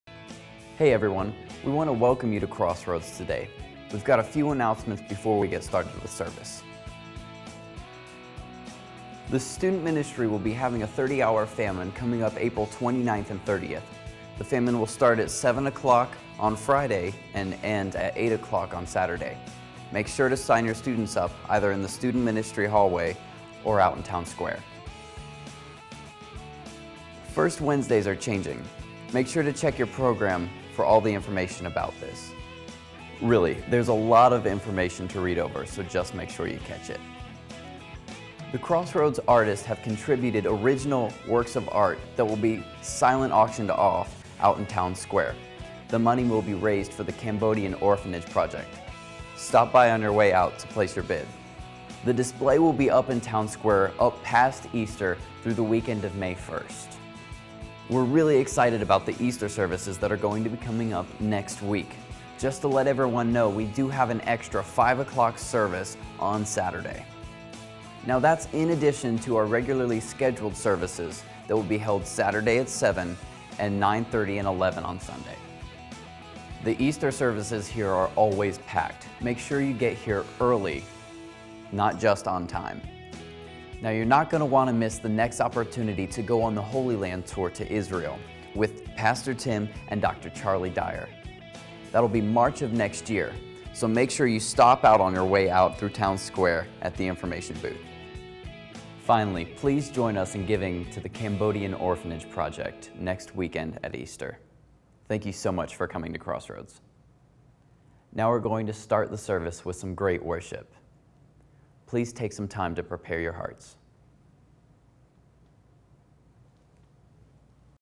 Palm Sunday announcements
Staying with our theme of re.purposed, I created our announcements this weekend with a stark white look. It matched the stage look as Jamie, one of our youth guys, told the congregation what is happening at Crossroads.